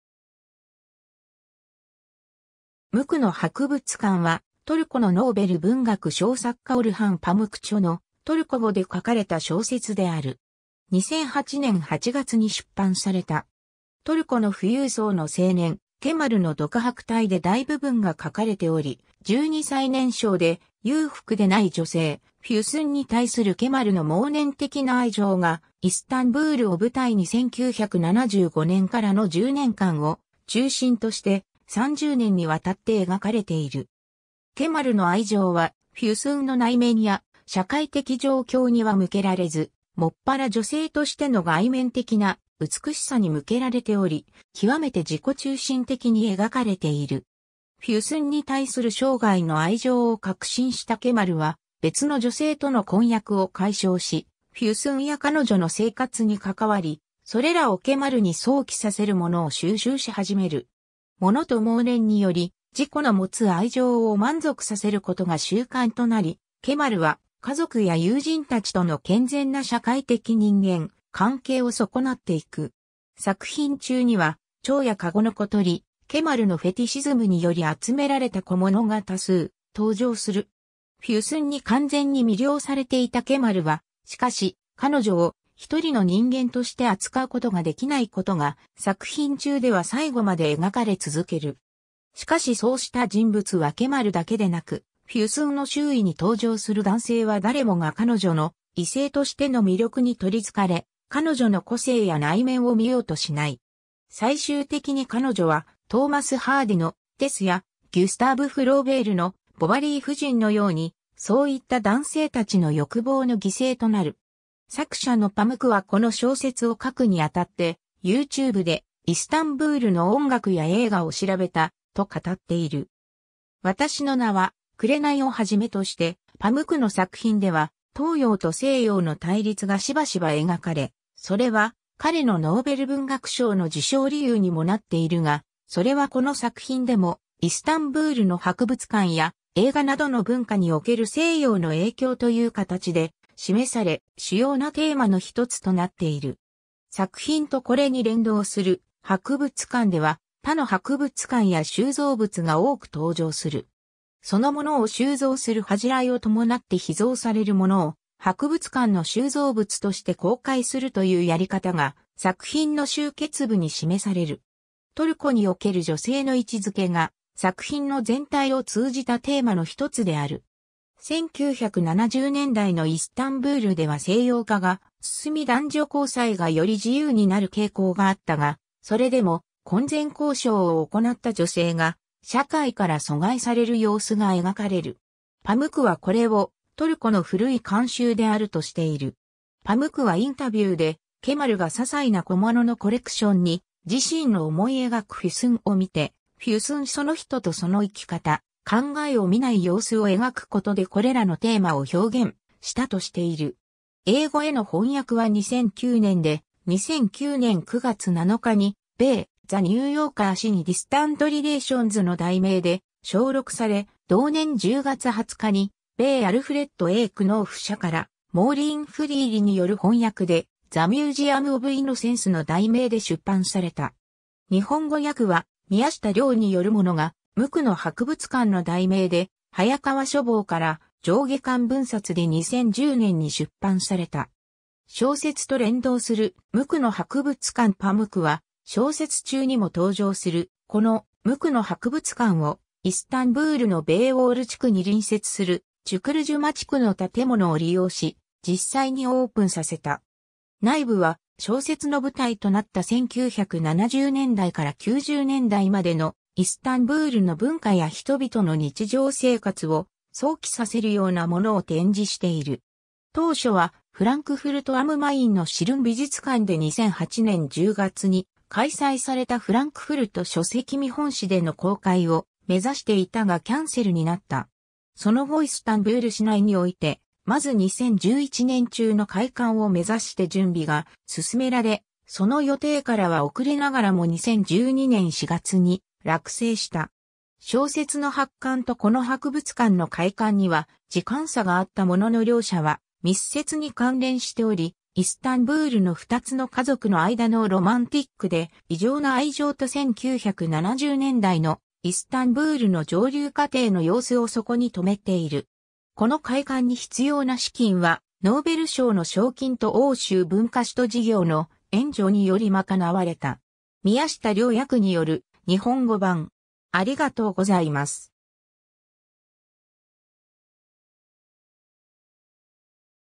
無垢の博物館
『無垢の博物館』（むくのはくぶつかん、トルコ語: Masumiyet Müzesi）は、トルコのノーベル文学賞作家オルハン・パムク著のトルコ語で書かれた小説である。
2008年8月に出版された（日本語訳は2010年）。
 トルコの富裕層の青年ケマルの独白体で大部分が書かれており、12歳年少で裕福でない女性フュスンに対するケマルの妄念的な愛情が、イスタンブールを舞台に1975年からの10年間を中心として、30年にわたって描かれている。
ケマルの愛情はフュスンの内面や社会的状況には向けられず、もっぱら女性としての外面的な美しさに向けられており、きわめて自己中心的に描かれている。
フュスンに対する生涯の愛情を確信したケマルは別の女性との婚約を解消し、フュスンや彼女の生活に関わり、それらをケマルに想起させる物を収集し始める。
物と妄念により自己の持つ愛情を満足させることが習慣となり、ケマルは家族や友人たちとの健全な社会的人間関係を損なっていく。
 作品中には蝶やカゴの小鳥、ケマルのフェティシズムにより集められた小物が多数登場する。
フュスンに完全に魅了されていたケマルは、しかし彼女をひとりの人間として扱うことができないことが、作品中では最後まで描かれ続ける。
 しかしそうした人物はケマルだけでなく、フュスンの周囲に登場する男性は誰もが彼女の異性としての魅力にとりつかれ、彼女の個性や内面を見ようとしない。
最終的に彼女は、トーマス・ハーディの「テス」やギュスターヴ・フローベールの「ボヴァリー夫人」のように、そういった男性たちの欲望の犠牲となる。
 作者のパムクはこの小説を書くに当って、YouTubeでイスタンブールの音楽や映画を調べた、と語っている。
 「わたしの名は紅」をはじめとしてパムクの作品では東洋と西洋の対立がしばしば描かれ、それは彼のノーベル文学賞の受賞理由にもなっているが、それはこの作品でも、イスタンブールの博物館や映画などの文化に於ける西洋（ヨーロッパ及びアメリカ）の影響という形で示され、主要なテーマの一つとなっている。
 作品とこれに連動する博物館では、他の博物館や収蔵物が多く登場する。
そのものを収蔵する恥じらいを伴って秘蔵されるものを博物館の収蔵物として公開するというやり方が、作品の終結部に示される。
 トルコにおける女性の位置づけが作品の全体を通じたテーマの一つである。
1970年代のイスタンブールでは西洋化が進み男女交際がより自由になる傾向があったが、それでも婚前交渉を行った女性が社会から阻害される様子が描かれる。
パムクはこれをトルコの古い慣習であるとしている。
 パムクはインタビューで、ケマルが些細な小物のコレクションに自身の思い描くフュスンを見て、フュスンその人とその生き方、考えを見ない様子を描くことでこれらのテーマを表現したとしている。
 英語への翻訳は2009年で、2009年9月7日に米『ザ・ニューヨーカー』誌に Distant Relations の題名で抄録され、同年10月20日に米Alfred A. Knopf社からモーリーン・フリーリによる翻訳で The Museum of Innocence の題名で出版された。
 日本語訳は宮下遼によるものが「無垢の博物館」の題名で、早川書房から上下巻分冊で2010年に出版された。
 小説と連動する「無垢の博物館」 パムクは小説中にも登場するこの「無垢の博物館」を、イスタンブールのベイオール地区 (Beyoğlu) に隣接するチュクルジュマ地区 (Çukurcuma) の建物を利用し実際にオープンさせた。
 内部は小説の舞台となった1970年代から90年代までのイスタンブールの文化や人々の日常生活を想起させるような物を展示している。
 当初はフランクフルト・アム・マインのシルン美術館 (en) で2008年10月に開催されたフランクフルト書籍見本市での公開を目指していたがキャンセルになった。
その後イスタンブール市内において、まず2011年中の開館を目指して準備が進められ、 その予定からは遅れながらも2012年4月に落成した。
小説の発刊とこの博物館の開館には時間差があったものの両者は密接に関連しており、イスタンブールの二つの家族の間のロマンティックで異常な愛情と1970年代のイスタンブールの上流家庭の様子をそこに留めている.。
この開館に必要な資金はノーベル賞の賞金と欧州文化首都事業 （2010年イスタンブール）の援助によりまかなわれた。...